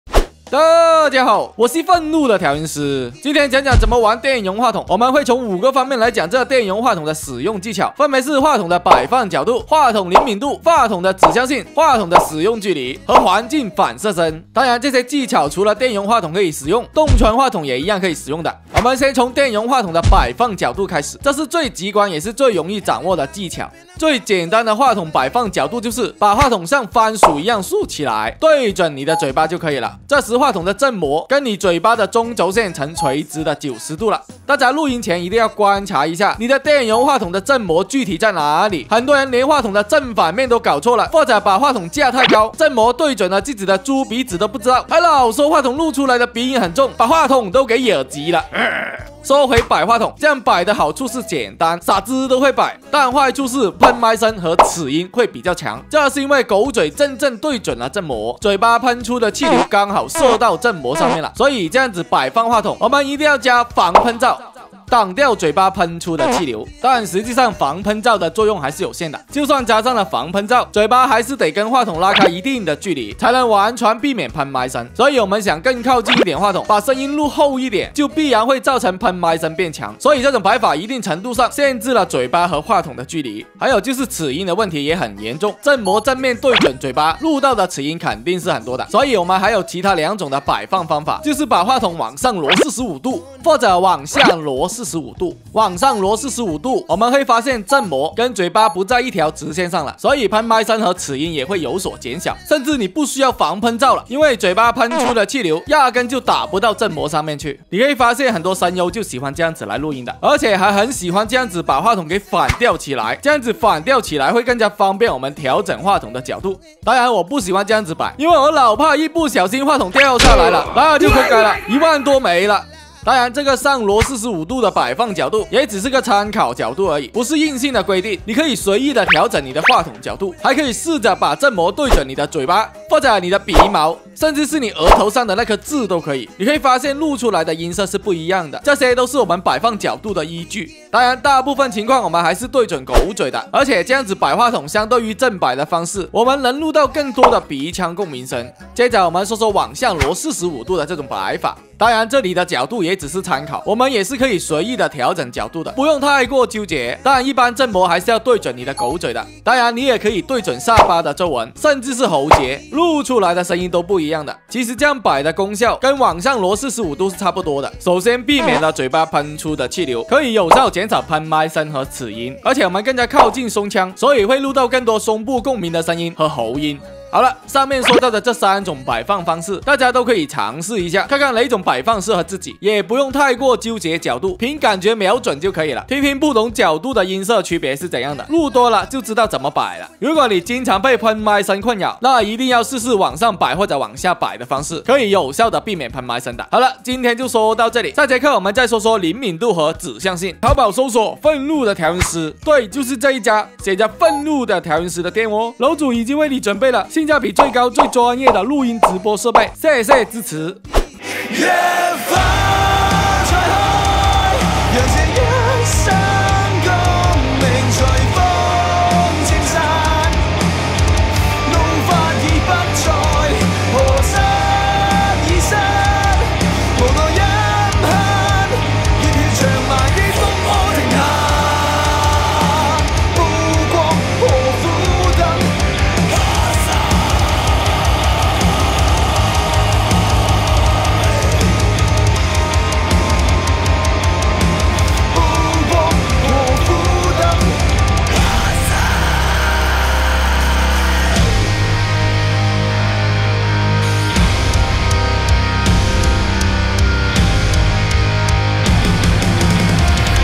主播必看 电容麦5招实用技巧：1集 话筒的摆放角度 从小白到大师 愤怒的调音师
某宝搜索：愤怒的调音师

楼主已经为你准备了最专业音频设备

保证绝对正品